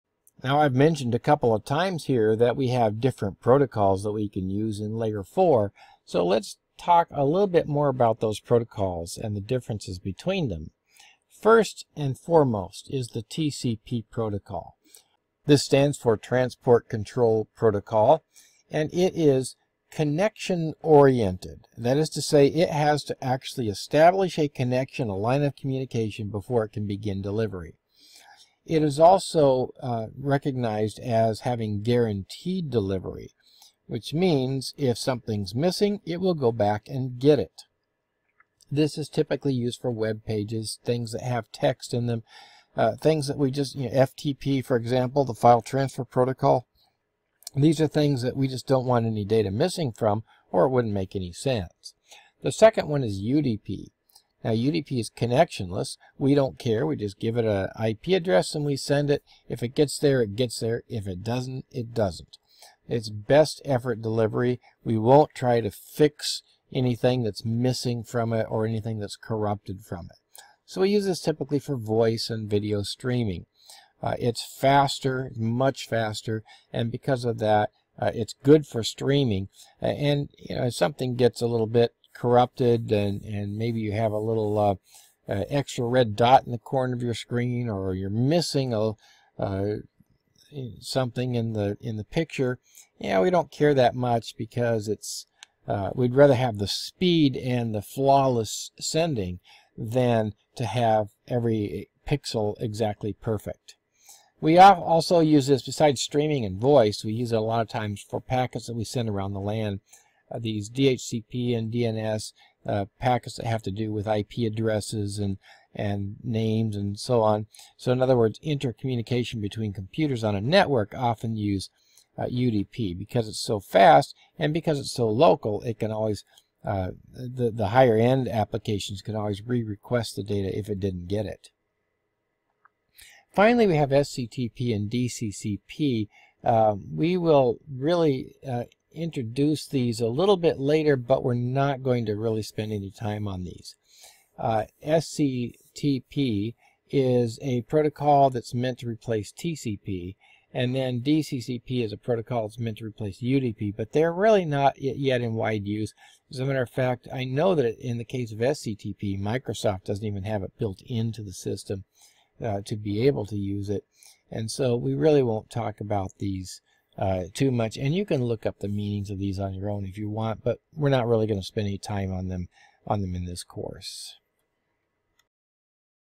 5-03 Layer 4 protocols
This video is part of an Intro to Networking class, which is a brief survey of many networking terms and concepts. It is not meant as an in-depth tutorial on this topic.
If you find inaccuracies, please let me know. I want to be sure they are as accurate as I can make them.